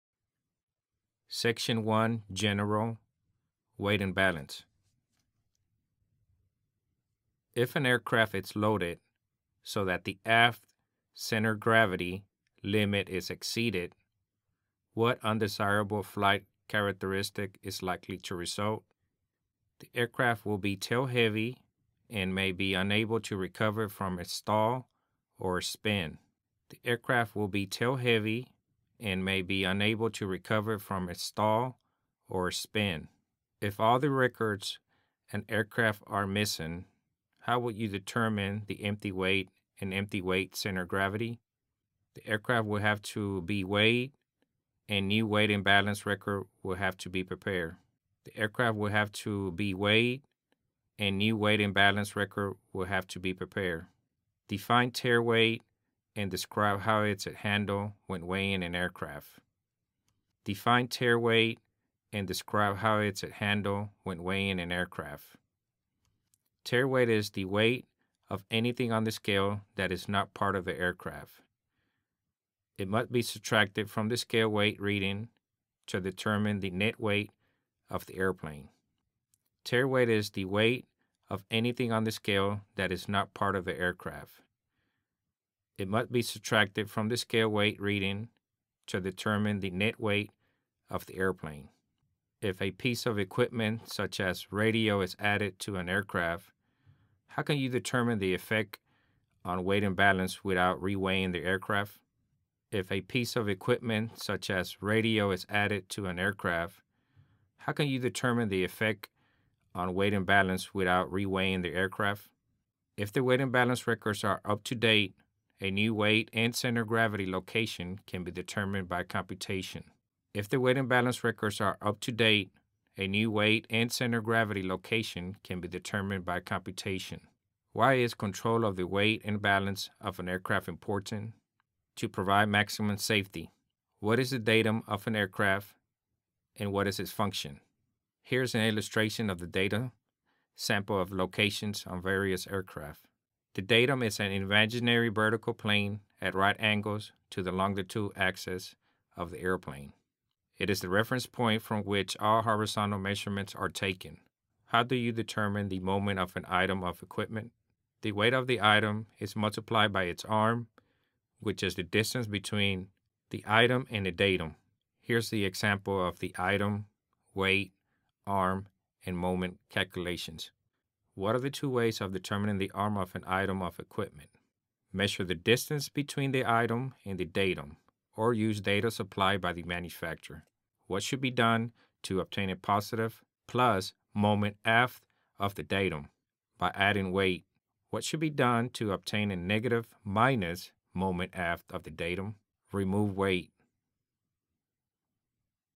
General : Weight and Balance Oral Example
Ref: FAA-H-8083-30

Objective: To determine that the applicant exhibits knowledge in the following elements:

a. the purpose(s) of weighing or reweighing.
b. general preparations for weighing, with emphasis on aircraft preparation and/or weighing area considerations.
c. the general location of airplane center of gravity (CG) in relation to the center of lift for most fixed main airfoils.
d. definitions of any of the following: datum, arm, moment (positive or negative), or moment index.
e. the meaning and/or application of any terms/nomenclature associated with weight and balance other than those mentioned in element “d” above, including but not limited to any of the following: tare, ballast, and residual fuel/oil.
f. procedures for finding any of the following: datum, arm, moment (positive or negative), or moment index.
g. purpose and/or application of mean aerodynamic chord (MAC).
h. adverse loading considerations.